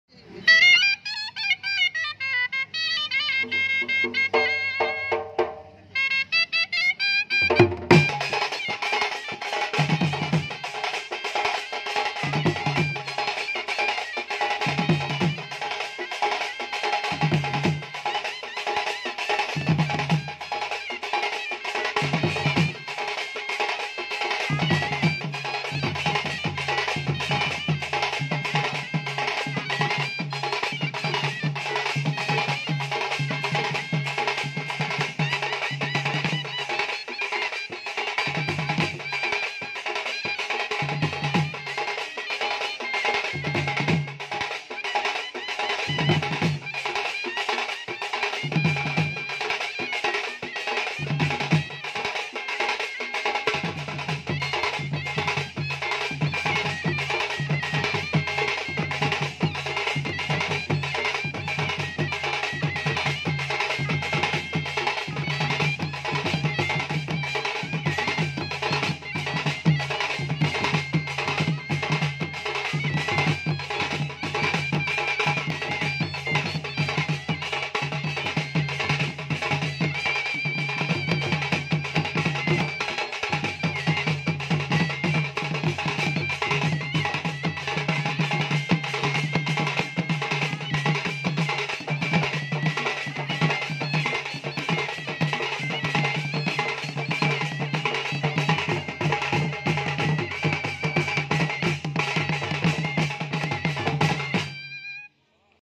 desi baja contact-9777760300
desi baja special the all rounder team artists in our balangir district all odisha artists

Name-muhuria Arjun nanada, famous dholia - kuldhar deep
famous tashawala - mantu nag
record Lishanwala- arjun bibhar, satyban barik
maind blowing - talwala - narayan#guru